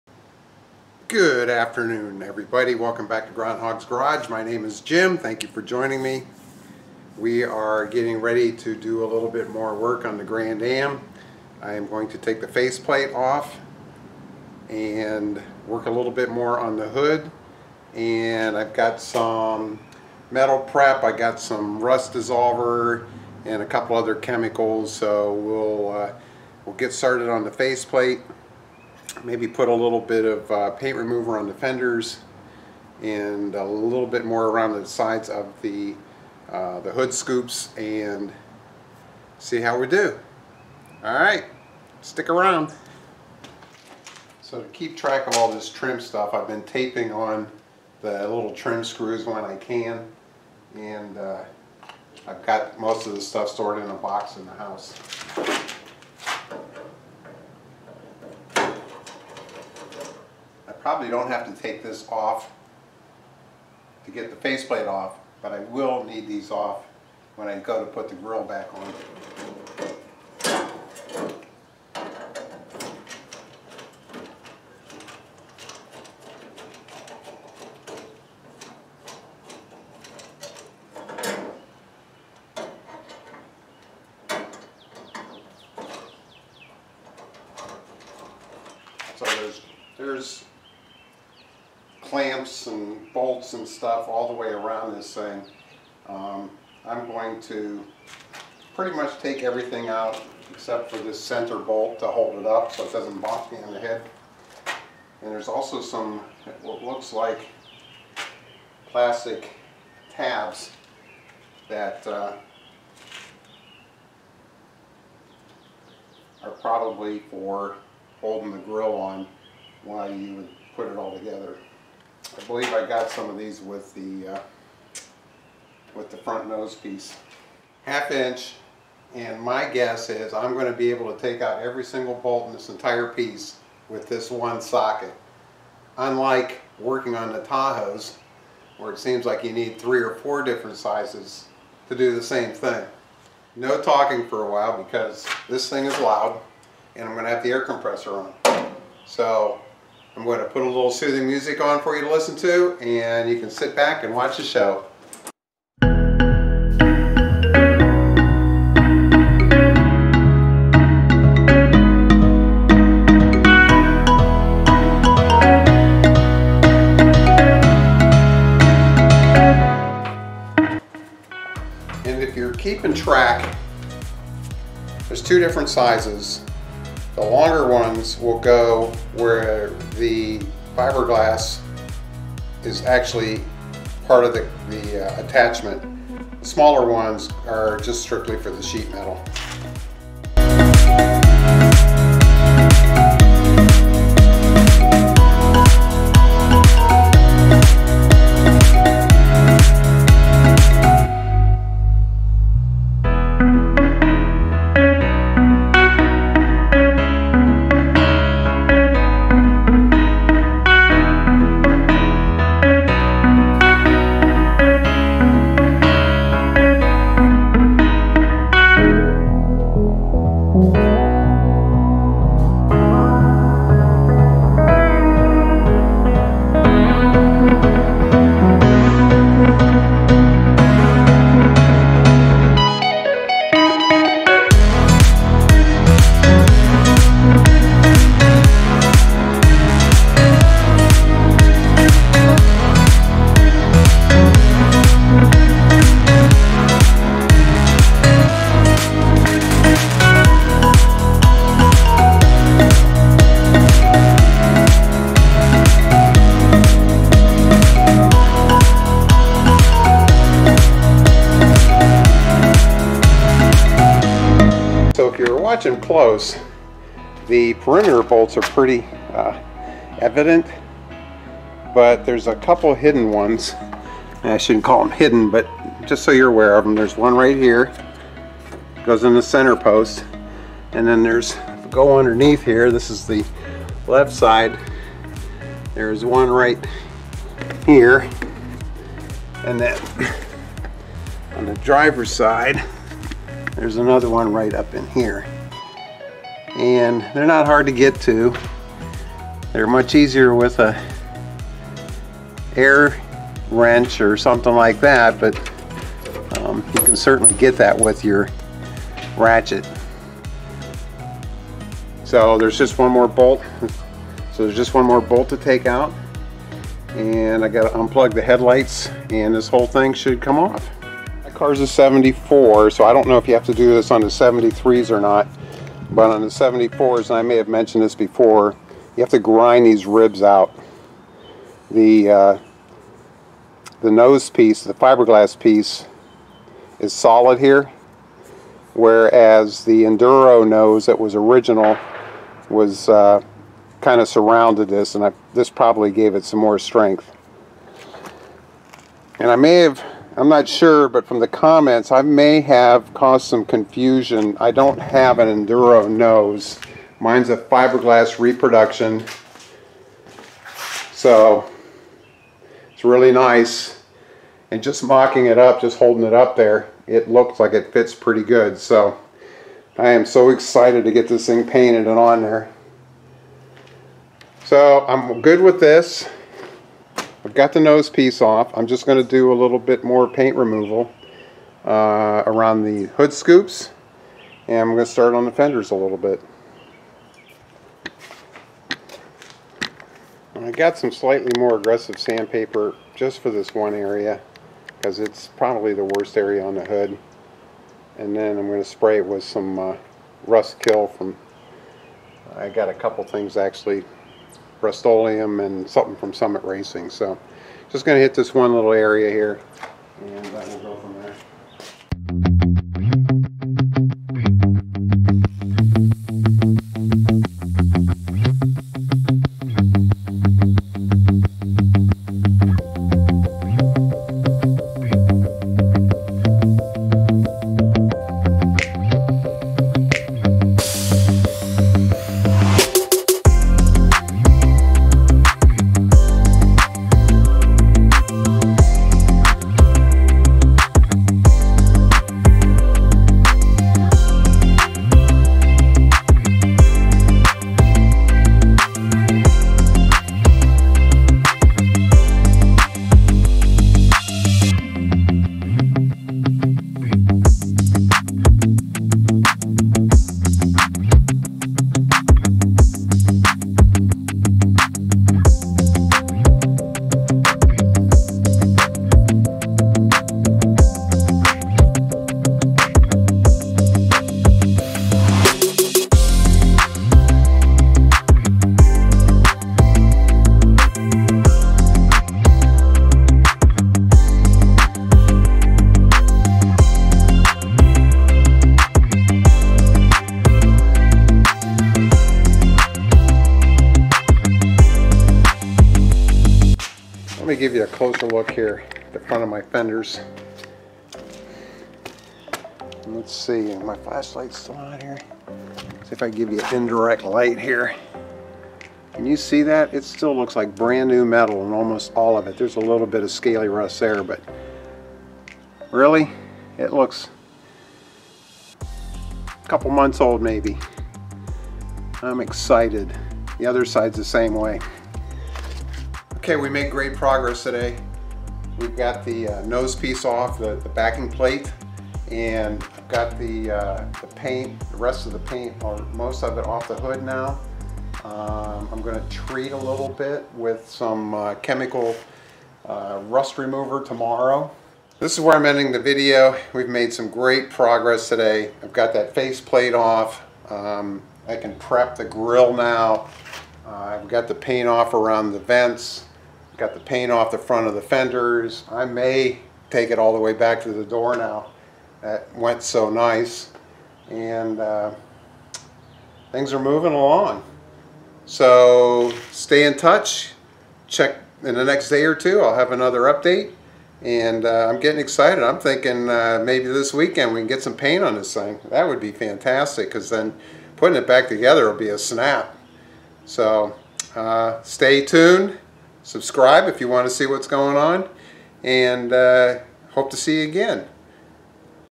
1974 Grand Am nose job 5
Continuing down the road of refurbishing the front clip on my 1974 Pontiac Grand Am.  It's slow going, but progress is being made.  In this video I take off the backing plate for the nose piece and continue working on the hood of the car.